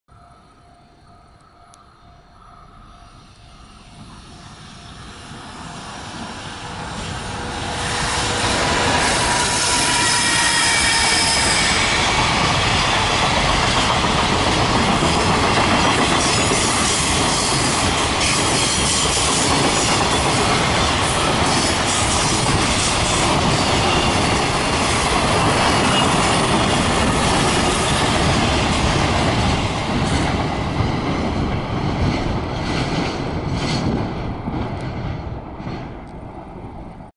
EF210-149号機[吹]代走牽引3075レ 安倍川駅～用宗駅間通過
浪速運送のU28Aコンテナ・・・
結構目立ちますね。
ちなみに改正で3075レの安倍川駅運転停車はなくなりました。